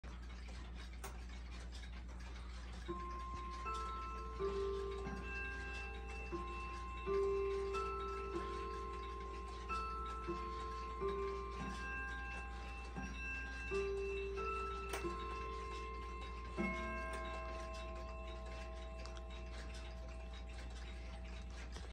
Howard Miller Sandringham Wall Clock Chimes 1 PM
Requester: Anonymous

Here we have probably my shortest video ever of the Howard Miller Sandringham. I started the video a few seconds too late as you could probably see… My fault.

Consider joining the Clock TV Discord Community! We are looking for new people to chat with!